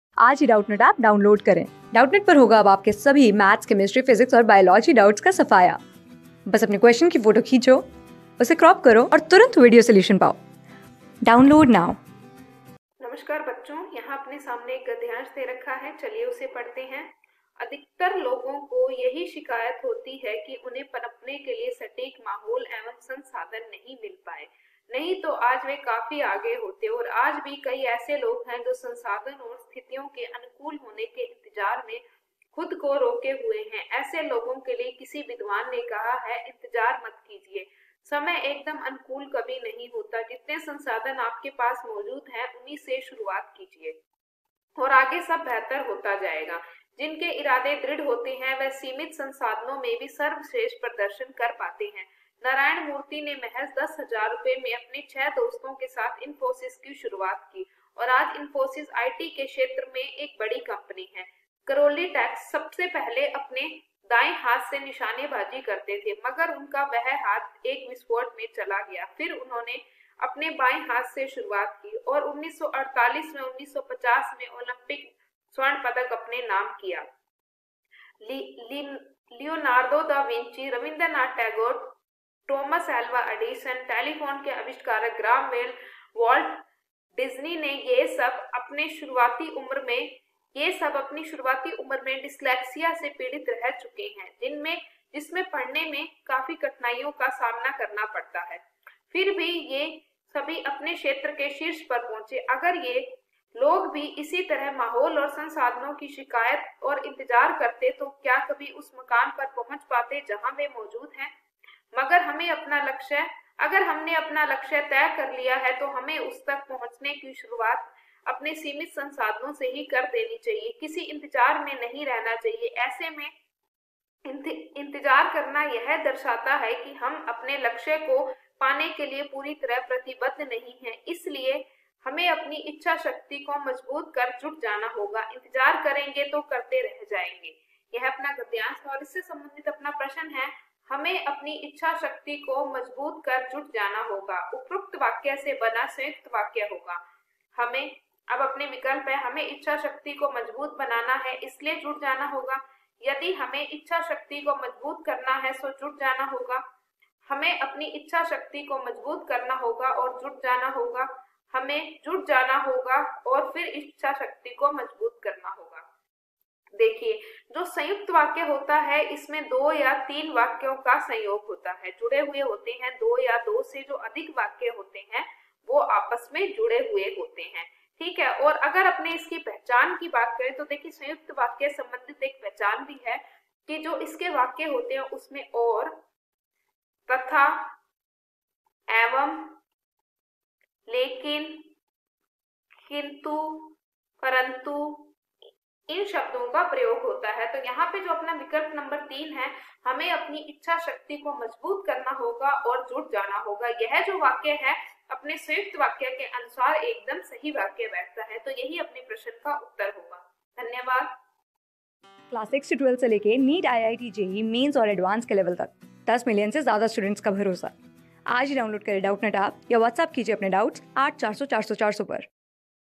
अधिकतर लोगों की यही शिकायत होती है कि उन्हें पनपने के लिए सटीक माहौल व संसाधन नहीं मिल पाए, नहीं...
अधिकतर लोगों की यही शिकायत होती है कि उन्हें पनपने के लिए सटीक माहौल व संसाधन नहीं मिल पाए, नहीं तो आज वे काफी आगे होते और आज भी ऐसे कई लोग हैं, जो संसाधन और स्थितियों के अनुकूल होने के इन्तजार में खुद को रोके हुए हैं। ऐसे लोगों के लिए ही किसी विद्वान ने कहा है- इन्तजार मत कीजिए, समय एकदम अनुकूल कभी नहीं होता। जितने संसाधन आपके पास मौजूद हैं, उन्हीं से शुरुआत कीजिए और आगे सब बेहतर होता जाएगा। जिनके इरादे दृढ़ होते हैं, वे सीमित संसाधनों में भी सर्वश्रेष्ठ प्रदर्शन कर पाते हैं। नारायणमूर्ति ने महज दस हजार रुपये में अपने छ: दोस्तों के साथ इन्फोसिस की शुरुआत की और आज इन्फोसिस आईटी के क्षेत्र की एक बड़ी कम्पनी है। करौली टैकस, पहले अपने दाएँ हाथ से निशानेबाजी करते थे, मगर उनका वह हाथ एक विस्फोट में चला गया। फिर उन्होंने अपने बाएँ हाथ से शुरुआत की और 1948 व 1950 में ओलम्पिक स्वर्ण पदक अपने नाम किया। लिओनार्दो द विची, रवीन्द्रनाथ टैगोर, टॉमस अल्वा एडिसन, टेलीफोन के आविष्कारक ग्राहम बेल, वॉल्ट डिज्नी-ये सब अपनी शुरुआती उम्र में डिस्लेक्सिया से पीड़ित रह चुके हैं, जिसमें पढ़ने में काफी कठिनाइयों का सामना करना पड़ता है, फिर भी ये सभी अपने-अपने क्षेत्र के शीर्ष पर पहुँचे। अगर ये लोग भी इसी तरह माहौल और संसाधनों की शिकायत और इन्तजार करते, तो क्या कभी उस मुकाम पर पहुँच पाते, जहाँ वे मौजूद है? अगर हमने अपना लक्ष्य तय कर लिया है, तो हमें उस तक पहुंचने की शुरुआत अपने सीमित संसाधनों से ही कर देनी चाहिए। किसी इन्तजार में नहीं रहना चाहिए। ऐसे में इन्तजार करना यह दर्शाता है कि हम अपने लक्ष्य को पाने के लिए पूरी तरह प्रतिबद्ध नहीं हैं। इसलिए हमें अपनी इच्छाशक्ति को मजबूत कर जुट जाना होगा। इन्तजार करेंगे, तो करते रह जाएंगे। \'हमें अपनी इच्छाशक्ति को मंजबूत कर जुट जाना होगा।\' उपयुक्त वाक्य से बना संयुक्त वाक्य होगा
Class: 14
Subject: HINDI
Chapter: अपठित गद्यांश
Board:CTET

👉 Have Any Query? Ask Us.